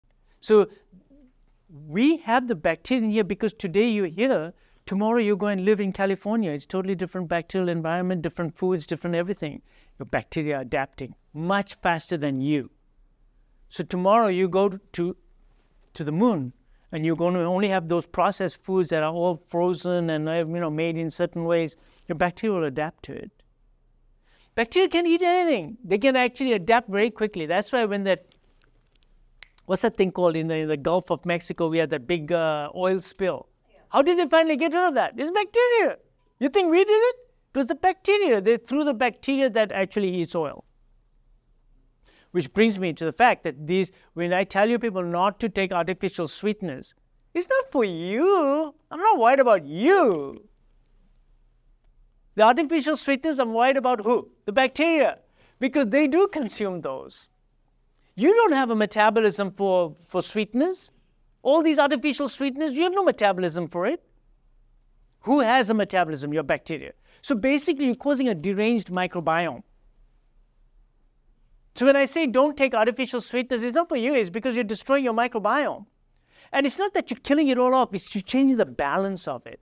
Bacteria Adapt to Everything? Artificial Sweeteners Ruin Gut!
Uncover the fascinating world of bacteria and their incredible adaptability! This video explores how these microscopic marvels thrive in diverse environments, from California to the moon, even tackling oil spills. Learn why artificial sweeteners disrupt your gut's delicate balance and the vital role bacteria play in our health.